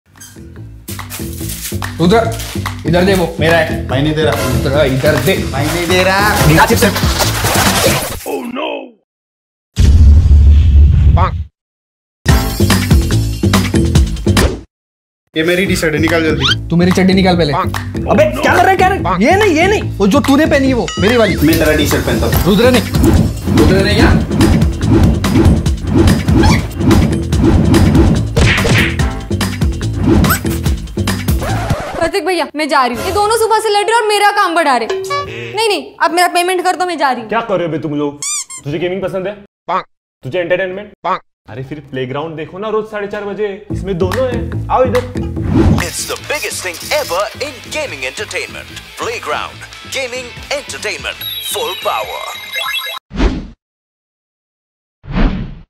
Part 26 - कामवाली बाई और भैया का झगड़ा 🤣 | Kaamwali Bai | Shorts Break
Go Check out and get yourself a FAADU T-shirt right away! Jab 2 Bachelors ke bich hua jhagda toh kaamwali bai kaise kare apna kaam.